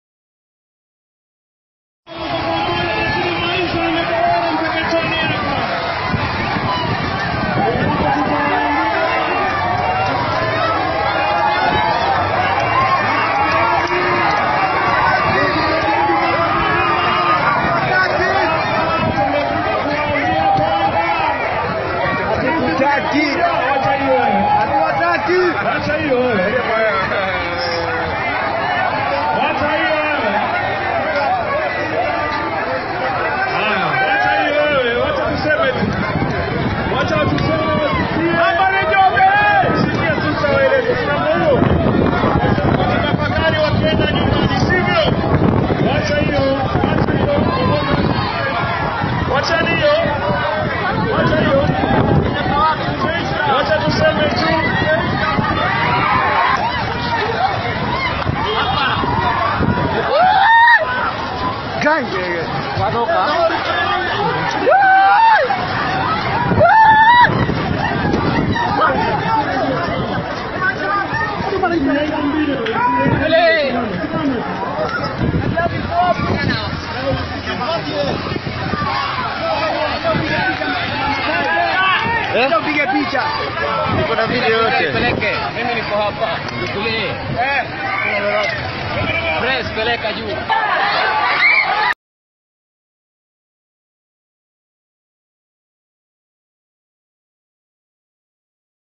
ODM leaders, Joho heckled in Mpeketoni
ODM team led by deputy Party leader Hassan Joho had to scamper for safety after youths in Mpeketoni, Lamu hurled stones and unripe mangoes at them.